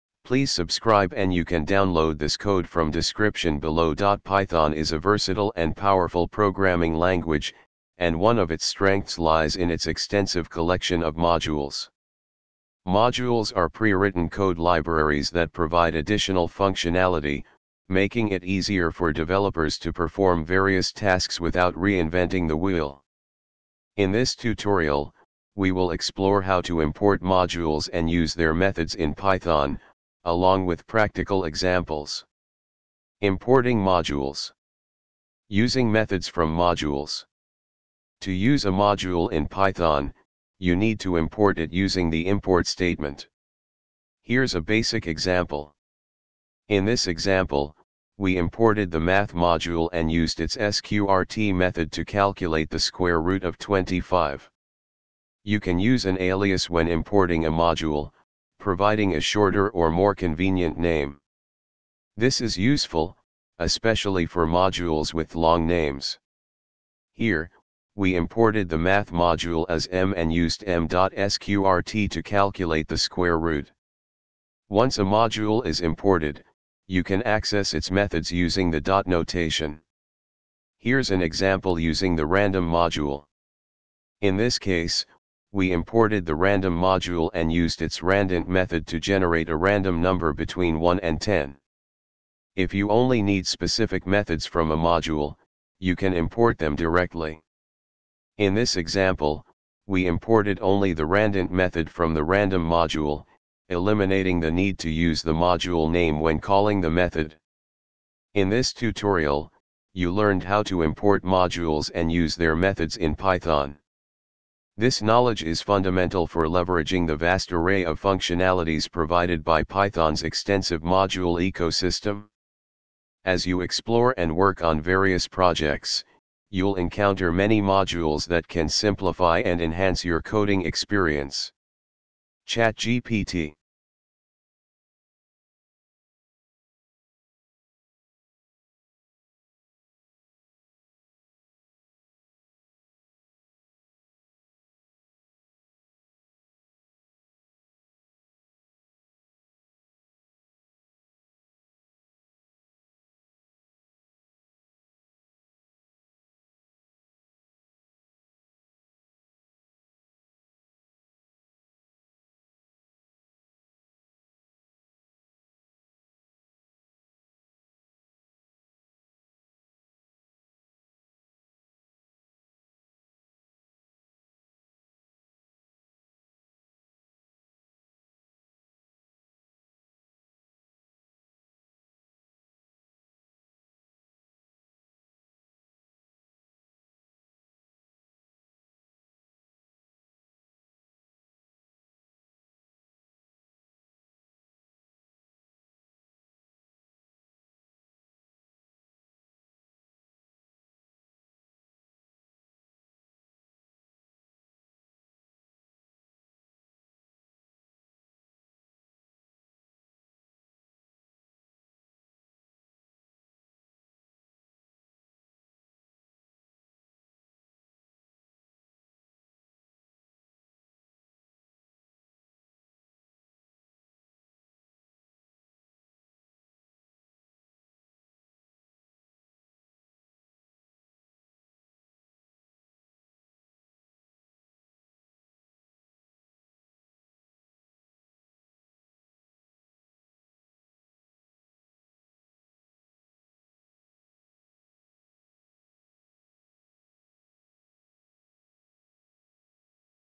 How to import module and use the methods in python
Python is a versatile and powerful programming language, and one of its strengths lies in its extensive collection of modules. Modules are pre-written code libraries that provide additional functionality, making it easier for developers to perform various tasks without reinventing the wheel. In this tutorial, we will explore how to import modules and use their methods in Python, along with practical examples.
Importing Modules
Using Methods from Modules
To use a module in Python, you need to import it using the import statement. Here's a basic example:
In this example, we imported the math module and used its sqrt method to calculate the square root of 25.
You can use an alias when importing a module, providing a shorter or more convenient name. This is useful, especially for modules with long names:
Here, we imported the math module as m and used m.sqrt to calculate the square root.
Once a module is imported, you can access its methods using the dot notation. Here's an example using the random module:
In this case, we imported the random module and used its randint method to generate a random number between 1 and 10.
If you only need specific methods from a module, you can import them directly:
In this example, we imported only the randint method from the random module, eliminating the need to use the module name when calling the method.
In this tutorial, you learned how to import modules and use their methods in Python. This knowledge is fundamental for leveraging the vast array of functionalities provided by Python's extensive module ecosystem. As you explore and work on various projects, you'll encounter many modules that can simplify and enhance your coding experience.
ChatGPT